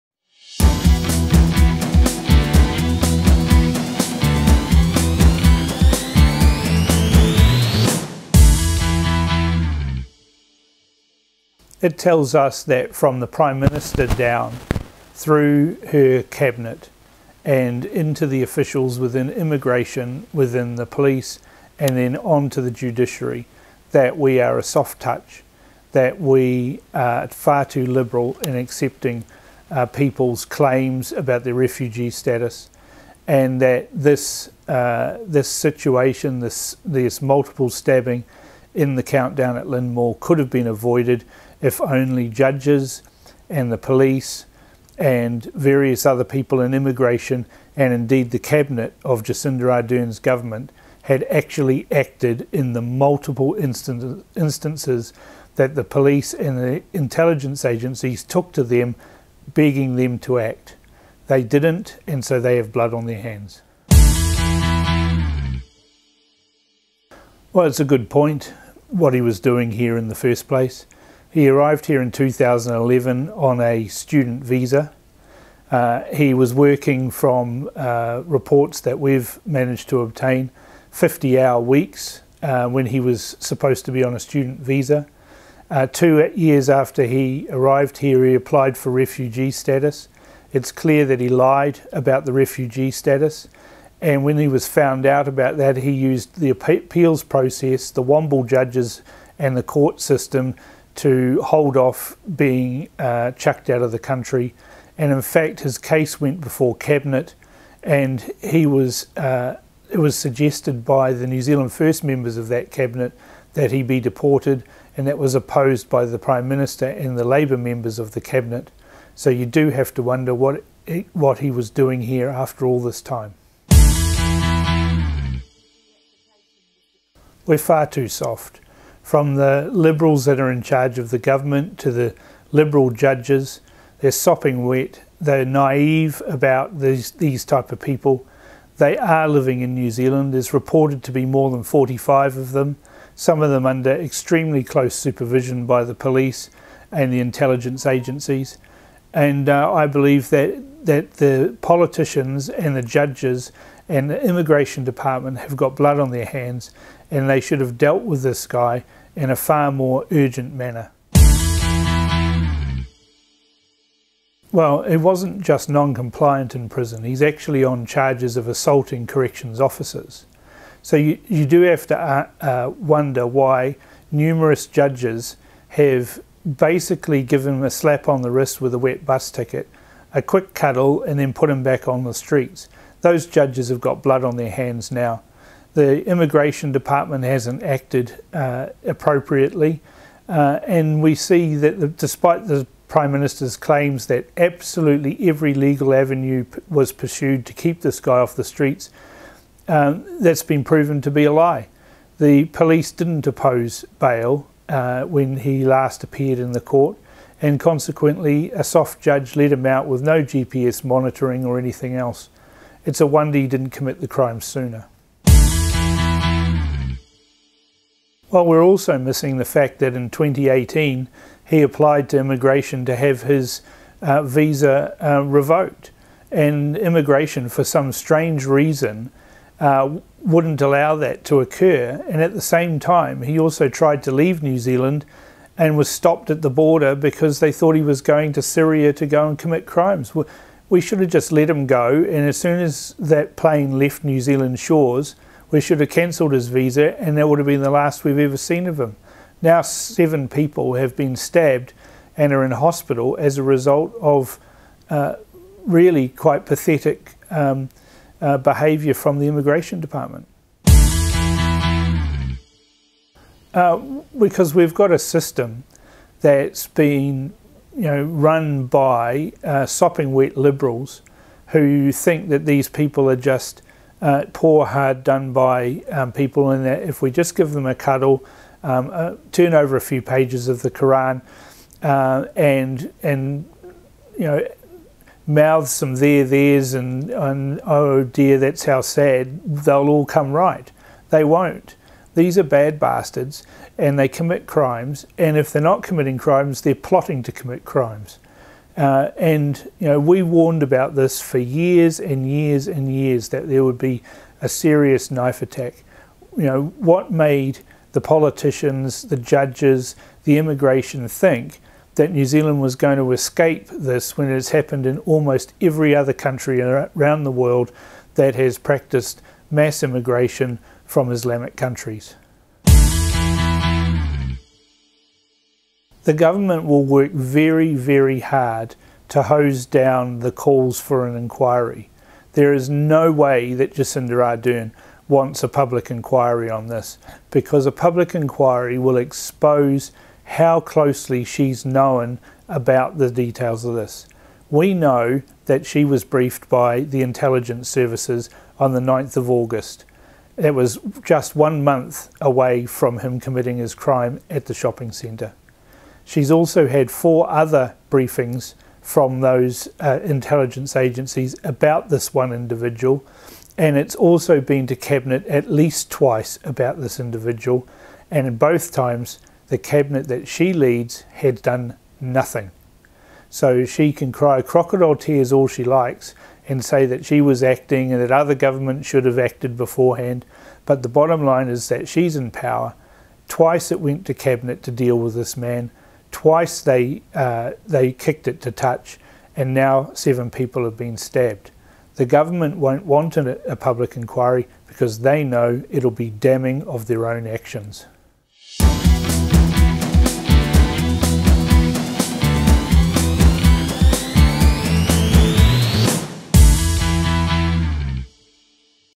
Politicast - Episode 5 - Lynnmall Terror Attack
Cam Slater discusses the latest terror attack in New Zealand by Ahamed Aathil Mohamed Samsudeen

This video is produced by Cam Slater and The BFD, New Zealand's most well known blogger and trusted news site.